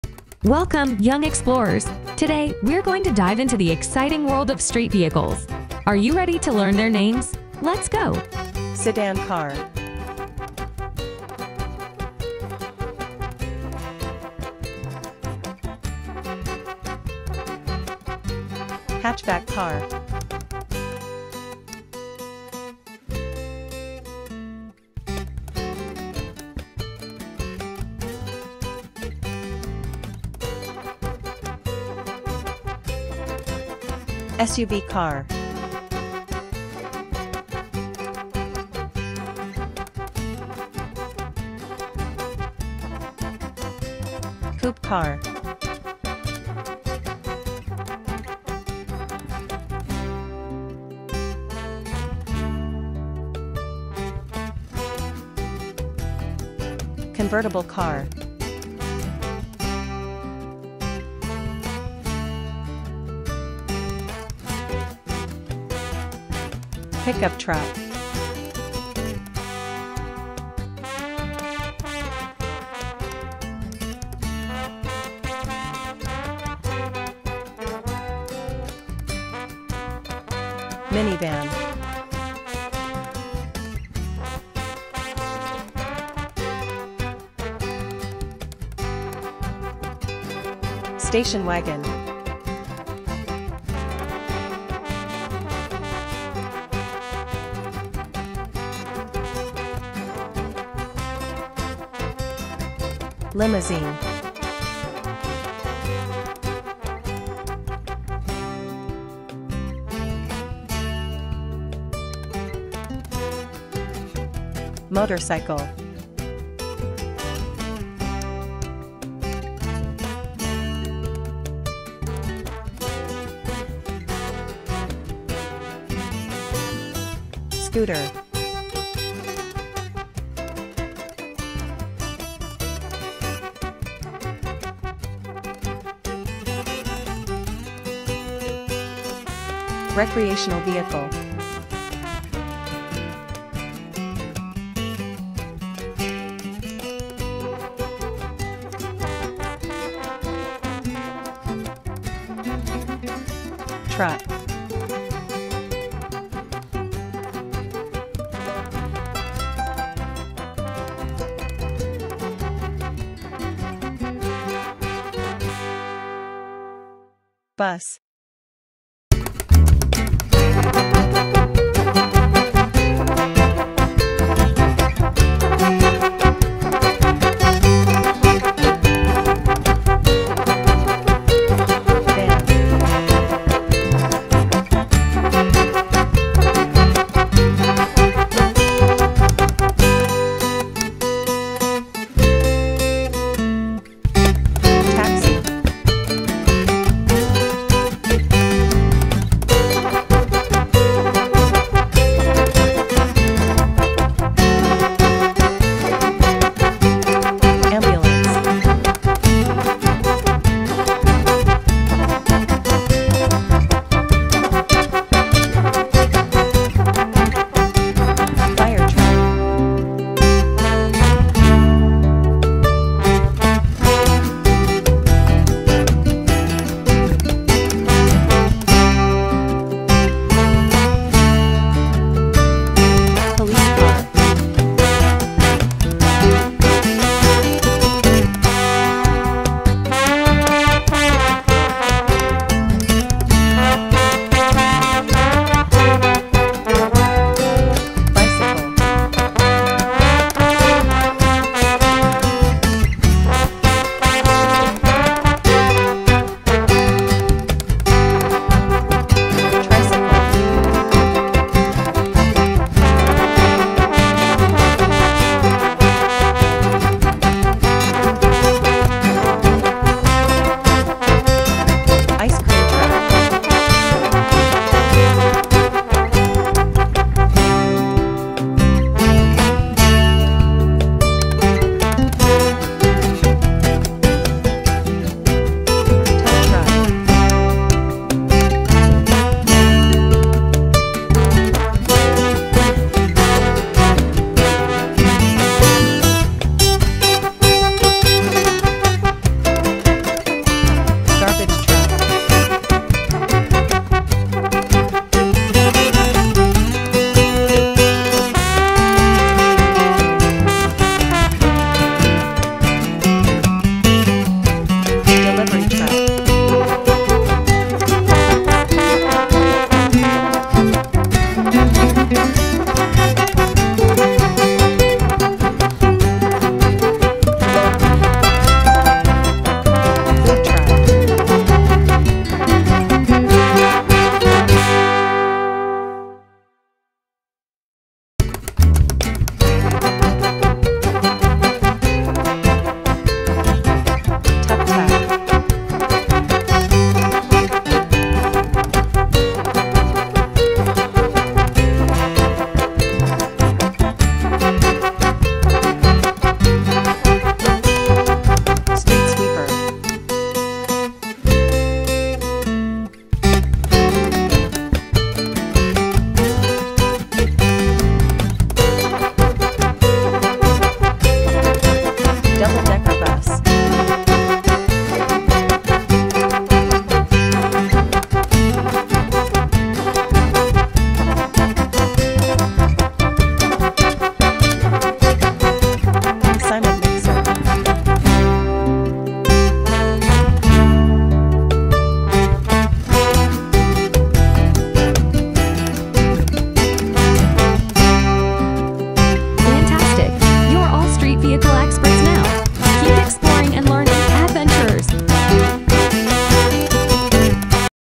Learning 30 Common Street Vehicles for Kids | Cars, Trucks, Fire Trucks, Garbage Trucks, & More
This animated cars & trucks video for kids teaches kids the names of 30 different street vehicles or city vehicles ( a Car, Minivan, Pickup Truck, SUV, Bus, School Bus, Garbage Truck, Street Sweeper, Fire Truck, Police Car, Ambulance, Tow Truck, Mail Truck, Delivery Truck, Taxi, Limousine, Motorcycle, Food Truck, and Ice Cream Truck ).  This fun, educational, learning kids cars & trucks video is ideal for children, toddlers, babies, special needs children, and children with learning disabilities, and is available to watch in full 4K.